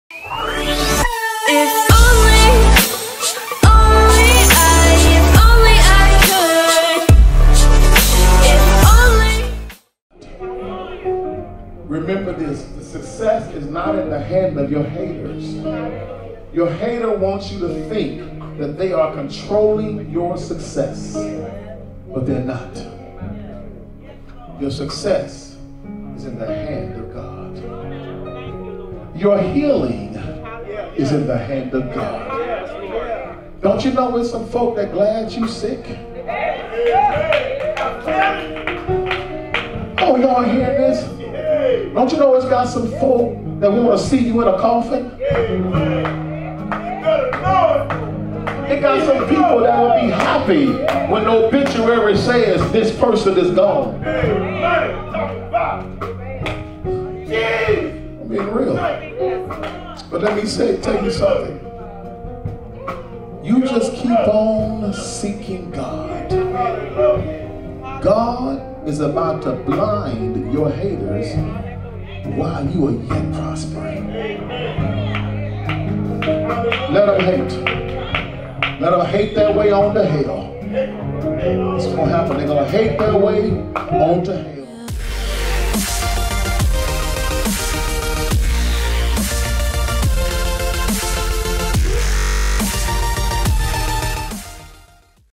Pastor Henry Williams Jr. Excerpt From (Hater In The House Sermon)
Shot By D.Fulton